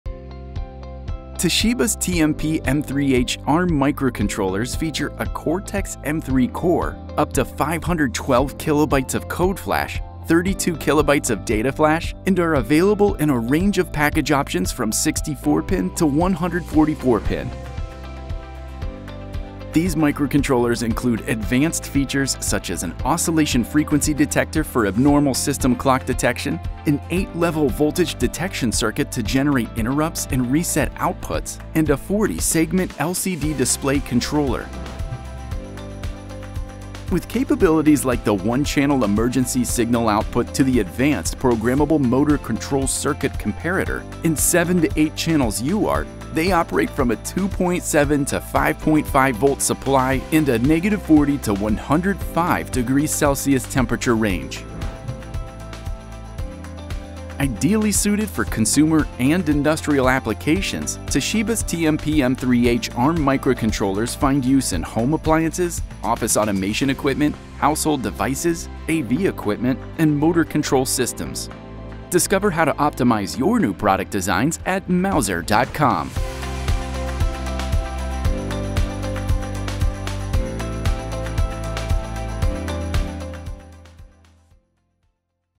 TOSHIBA TMPM3H Arm® Microcontroller - New Product Brief | Mouser Electronics
Introducing Toshiba's TMPM3H Arm Microcontrollers, now available at Mouser Electronics. In our "New Product Brief" video, we'll explore the incredible features and capabilities of these microcontrollers, providing you with valuable insights for your next project.

The TMPM3H Arm Microcontrollers from Toshiba are powered by a Cortex M3 core and come equipped with up to 512 kilobytes of code flash and 32 kilobytes of data flash. They offer a wide range of package options, ranging from 64-pin to 144-pin, ensuring that you can find the perfect fit for your application.

What sets these microcontrollers apart are their advanced features. They include an oscillation frequency detector for detecting abnormal system clock conditions, an eight-level voltage detection circuit for generating interrupts and reset outputs, and a 40-segment LCD display controller, opening up a world of possibilities for your designs.

These microcontrollers are incredibly versatile, offering capabilities like one-channel emergency signal output, an advanced programmable motor control circuit comparator, and seven to eight UART channels. They operate seamlessly within a supply voltage range of 2.7 to 5.5 volts and can withstand temperatures ranging from -40°C to 105°C.

Toshiba's TMPM3H ARM Microcontrollers are well-suited for a wide range of applications, including consumer and industrial sectors. You'll find them in home appliances, office automation equipment, household devices, AV equipment, and motor control systems, making them an ideal choice for a variety of projects.

Join us in our "New Product Brief" video as we dive deeper into the features, specifications, applications, and more of Toshiba's TMPM3H Arm Microcontrollers. Discover how these microcontrollers can empower your new product designs and bring your innovations to life.